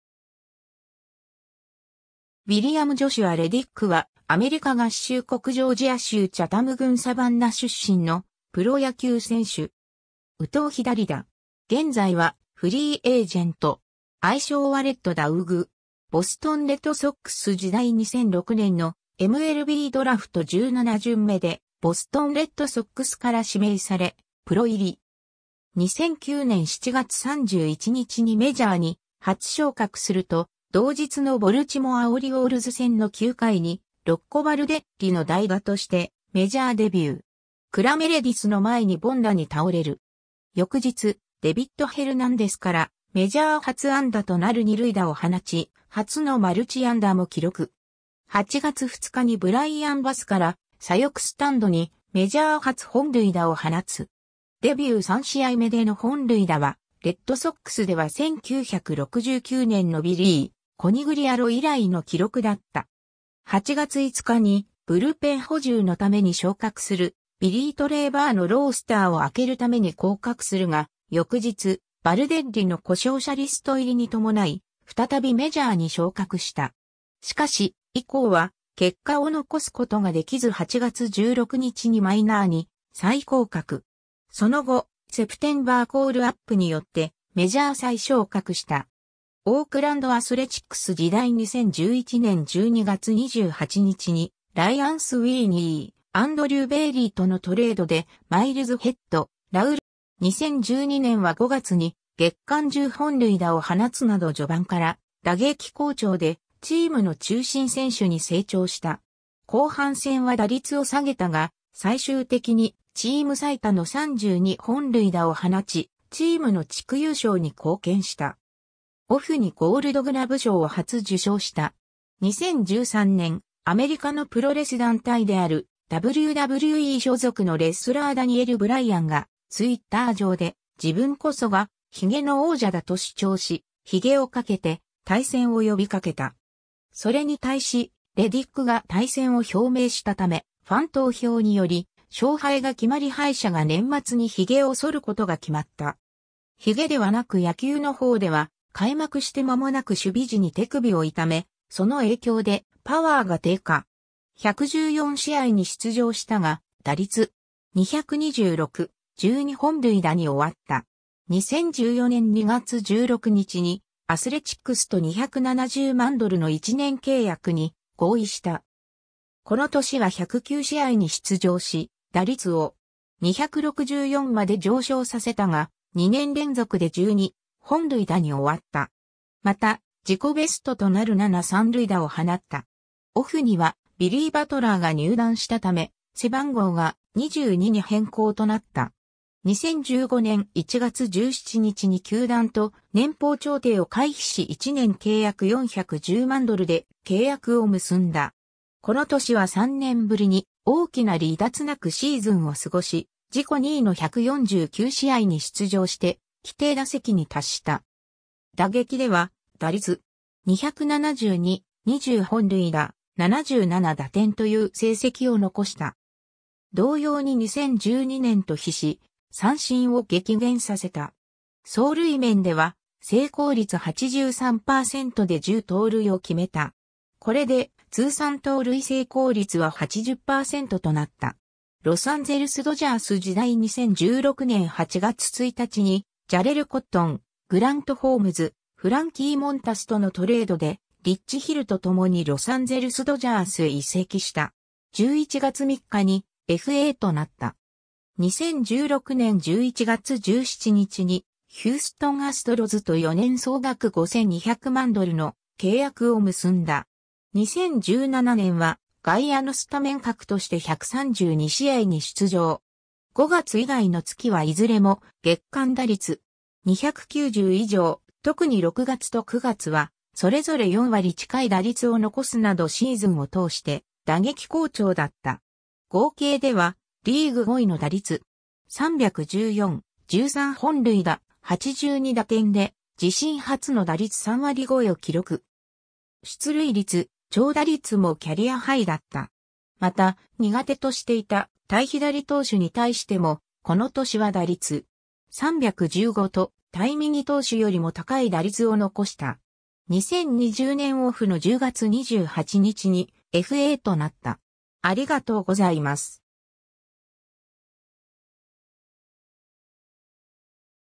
ジョシュ・レディック
ウィリアム・ジョシュア・レディック（William Joshua Reddick, 1987年2月19日 - ）は、アメリカ合衆国ジョージア州チャタム郡サバンナ出身のプロ野球選手（外野手）。
右投左打。
現在は、フリーエージェント（FA）。
愛称はレッド・ダウグ。
 ボストン・レッドソックス時代 （2011年7月20日） 2006年のMLBドラフト17巡目（全体523位）でボストン・レッドソックスから指名され、プロ入り。
 2009年7月31日にメジャーに初昇格すると、同日のボルチモア・オリオールズ戦の9回にロッコ・バルデッリの代打としてメジャーデビュー。
クラ・メレディスの前に凡打に倒れる。
翌日、デビッド・ヘルナンデスからメジャー初安打となる二塁打を放ち、初のマルチ安打も記録。
 8月2日にブライアン・バスから左翼スタンドにメジャー初本塁打を放つ。
デビュー3試合目での本塁打は、レッドソックスでは1969年のビリー・コニグリアロ以来の記録だった。
 8月5日にブルペン補充のために昇格するビリー・トレーバーのロースターを空けるために降格するが、翌日、バルデッリの故障者リスト入りに伴い、再びメジャーに昇格した。
しかし、以降は結果を残すことができず8月16日にマイナーに再降格。
その後、セプテンバー・コールアップによってメジャー再昇格した。
 オークランド・アスレチックス時代 （2015年5月25日） 2011年12月28日にライアン・スウィーニー、アンドリュー・ベイリーとのトレードで、マイルズ・ヘッド、ラウル・アルカンタラと共にオークランド・アスレチックスへ移籍した。
 2012年は5月に月間10本塁打を放つなど序盤から打撃好調で、チームの中心選手に成長した。
後半戦は打率を下げたが、最終的にチーム最多の32本塁打を放ち、チームの地区優勝に貢献した。
オフにゴールドグラブ賞（右翼手部門）を初受賞した。
 2013年、アメリカのプロレス団体であるWWE所属のレスラーダニエル・ブライアンがTwitter上で「自分こそがヒゲの王者だ」と主張し、ヒゲをかけて対戦を呼びかけた。
それに対しレディックが対戦を表明したため、ファン投票により勝敗が決まり敗者が年末にヒゲを剃る事が決まった。
ヒゲではなく野球の方では、開幕して間もなく守備時に手首を痛め、その影響でパワーが低下。
114試合に出場したが、打率.226、12本塁打に終わった。
 2014年2月16日にアスレチックスと270万ドルの1年契約に合意した。
この年は109試合に出場し、打率を.264まで上昇させたが、2年連続で12本塁打に終わった。
また、自己ベストとなる7三塁打を放った。
オフには、ビリー・バトラーが入団したため背番号が「22」に変更となった。
 2015年1月17日に球団と年俸調停を回避し1年契約410万ドルで契約を結んだ。
この年は3年ぶりに大きな離脱なくシーズンを過ごし、自己2位の149試合に出場して規定打席に達した。
打撃では、打率.272、20本塁打、77打点という成績を残した。
同様に2012年と比し、三振を激減させた。
走塁面では成功率83%で10盗塁を決めた。
これで、通算盗塁成功率は80%となった。
 ロサンゼルス・ドジャース時代 （2016年10月22日） 2016年8月1日にジャレル・コットン、グラント・ホームズ、フランキー・モンタスとのトレードで、リッチ・ヒルと共にロサンゼルス・ドジャースへ移籍した。
11月3日にFAとなった。
 2016年11月17日にヒューストン・アストロズと4年総額5200万ドルの契約を結んだ。
 2017年は外野のスタメン格として132試合に出場。
5月以外の月はいずれも月間打率.290以上、特に6月と9月はそれぞれ4割近い打率を残すなどシーズンを通して打撃好調だった。
合計ではリーグ5位の打率.314、13本塁打、82打点で、自身初の打率3割超えを記録。
出塁率、長打率もキャリアハイだった。
また、苦手としていた対左投手に対しても、この年は打率.315と対右投手よりも高い打率を残した。
 2020年オフの10月28日にFAとなった。